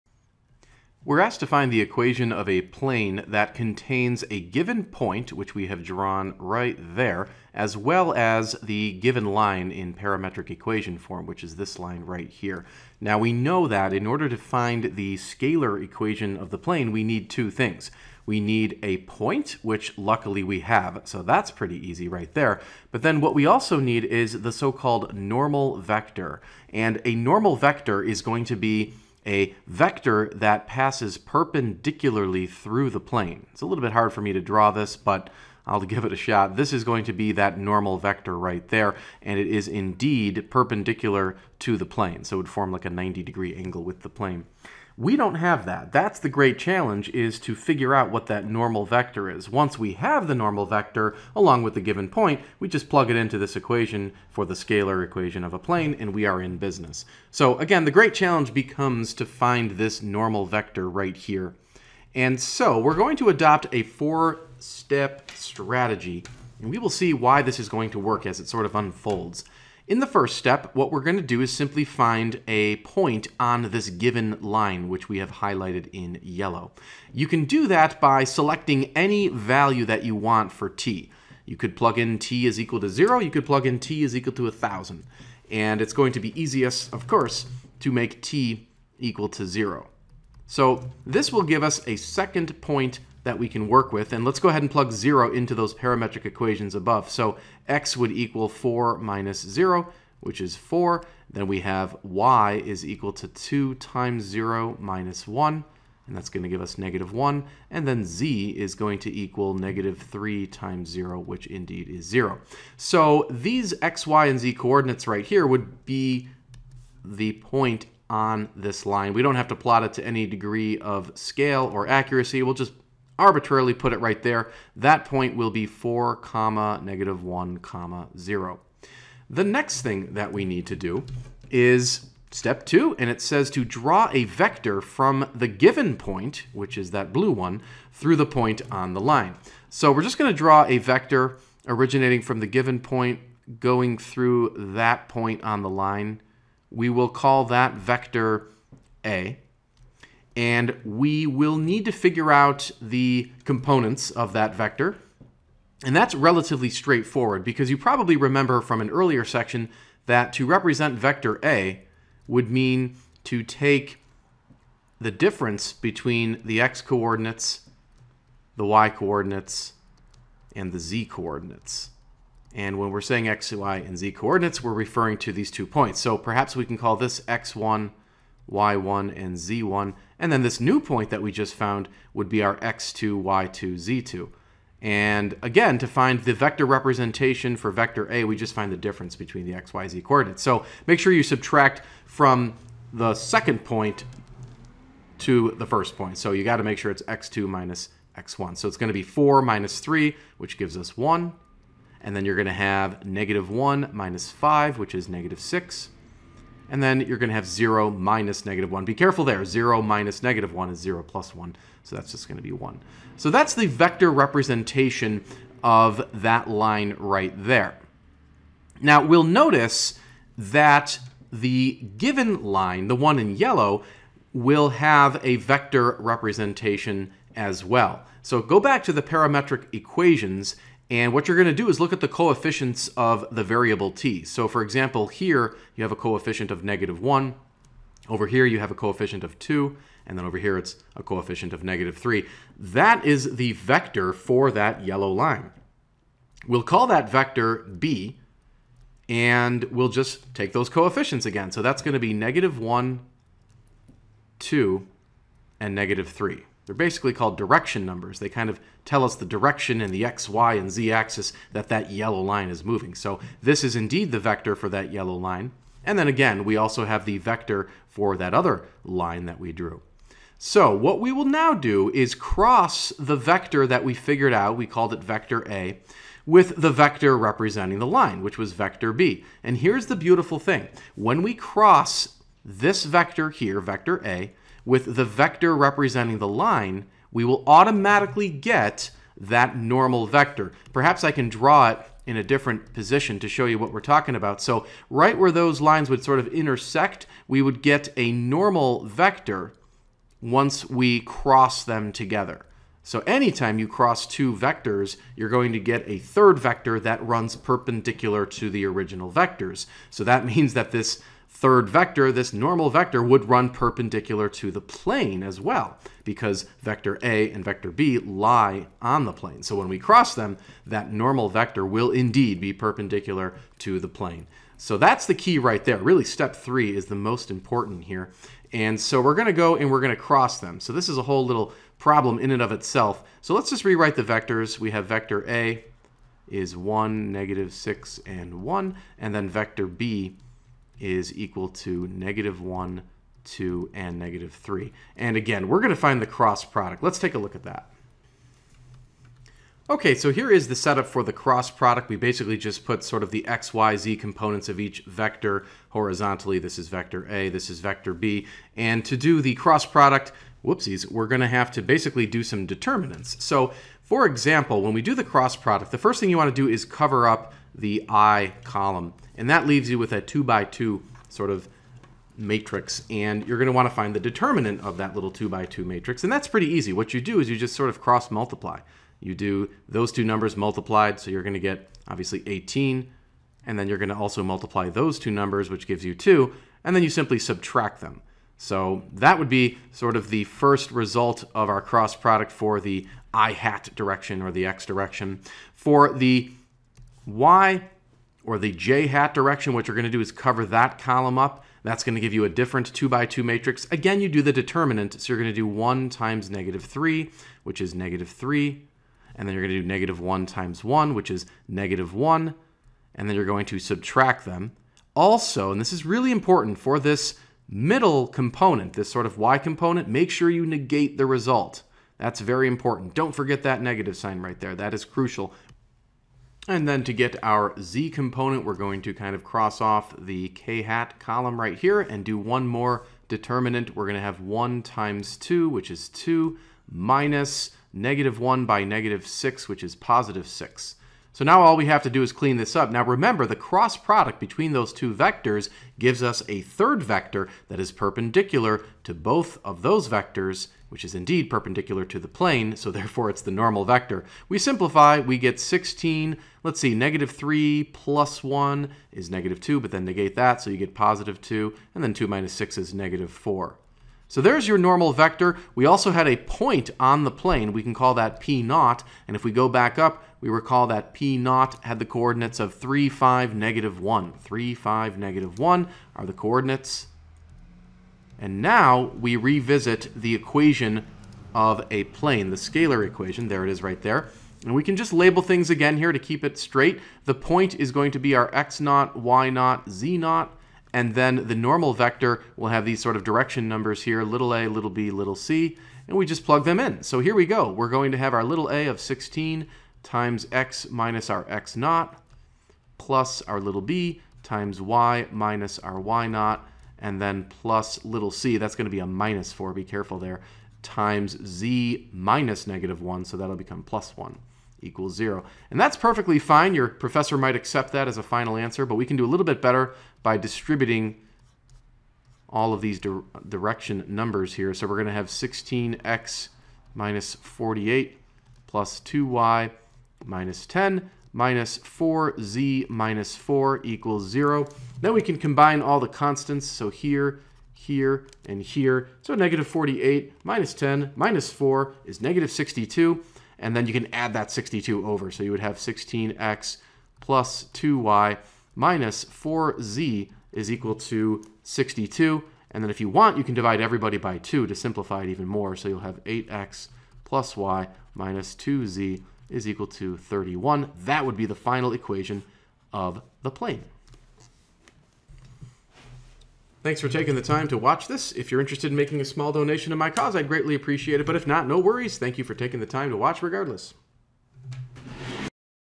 The plane that passes through the point and contains the line
Find an equation of the plane. The plane that passes through the point (3, 5, -1) and contains the line x = 4 - t, y = 2t - 1, z = -3t